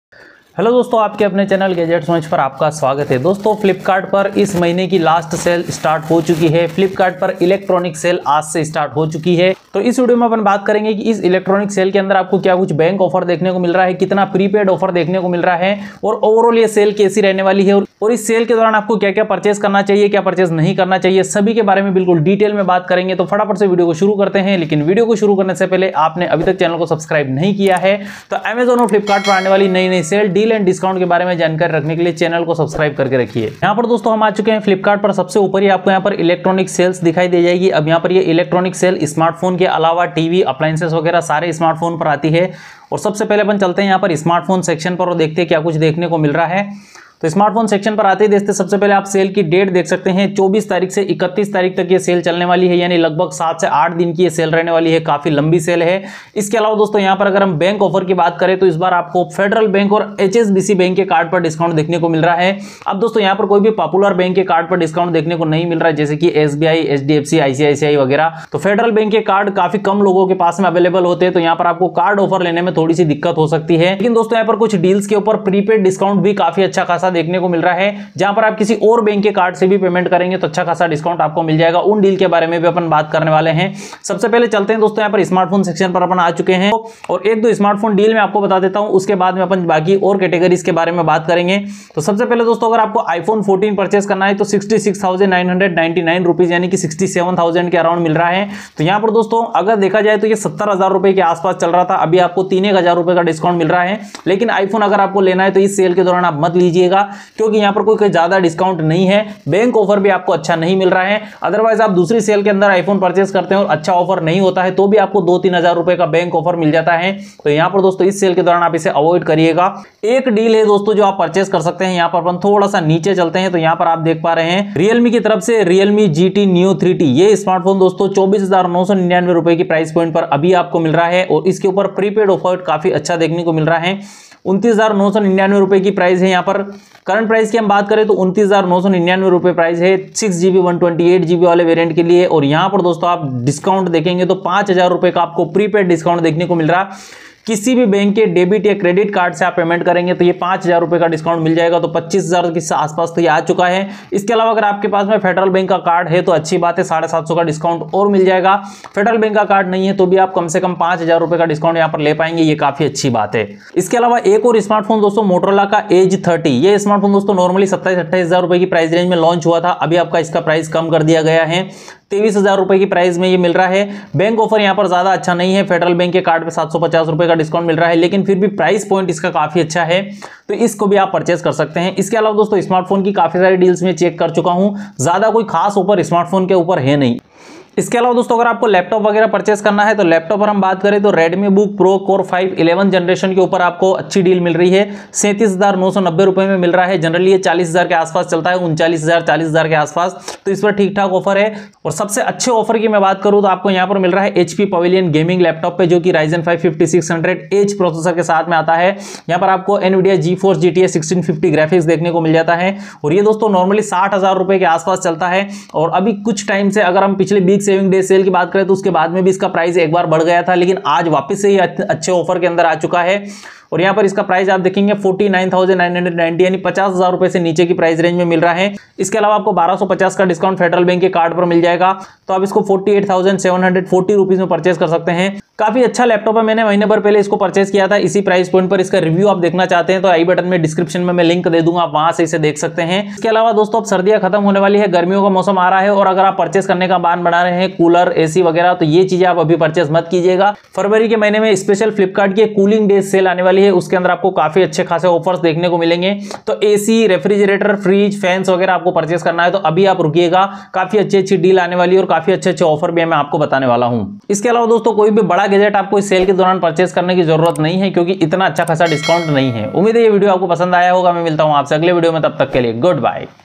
Flipkart Electronic Sale Offers Deals buy or not Upcoming Sale On Flipkart
1. Digitech Tripod
2. Boya Mic
3. Ring Light
4. Wireless Keyboard and Mouse
5 Powerbank
6. Earphones
7. Pendrive